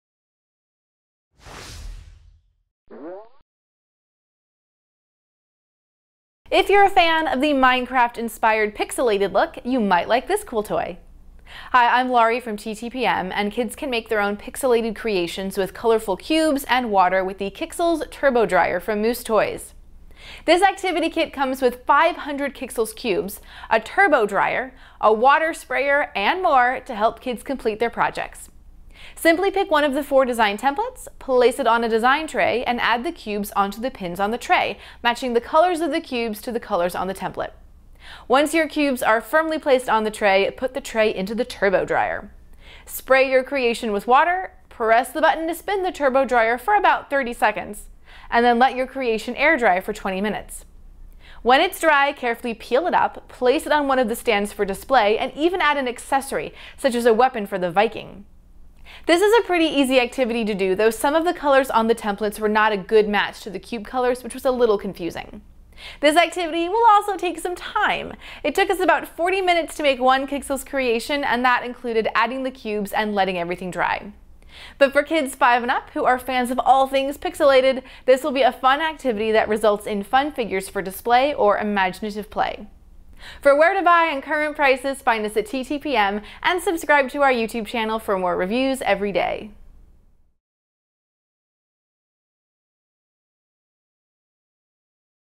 Qixels Turbo Dryer from Moose Toys
The Qixels Turbo Dryer takes the pixilated trend started by Minecraft and turns it into a fun and creative activity for boys. For full review and shopping info

Product Info:
The Minecraft-inspired pixilated look continues to be a pop culture trend, and kids can make their own pixilated creations with colorful cubes and water with the Qixels Turbo Dryer. This activity kit comes with 500 Qixels cubes, four design templates, a turbo dryer, two design trays, two display stands, two display bases, two plug-in accessories, a bag tag and thread, a water sprayer, and instructions. Simply pick which design template you want to use (dinosaur, Viking, plane, or scorpion), place it on a design tray, and add the cubes onto the pins on the tray, matching the colors of the cubes to the colors on the template. Once the cubes are firmly placed on the tray, put the tray into the turbo dryer. Spray the creation with water, press the button to spin the turbo dryer for about 30 seconds, and then let your creation air-dry for 20 minutes. When it's dry, carefully peel it up, place it on one of the stands for display, and even add an accessory, such as a weapon for the Viking. Also make sure to remove the drainage tray from the side of the turbo dryer and pour out any captured water.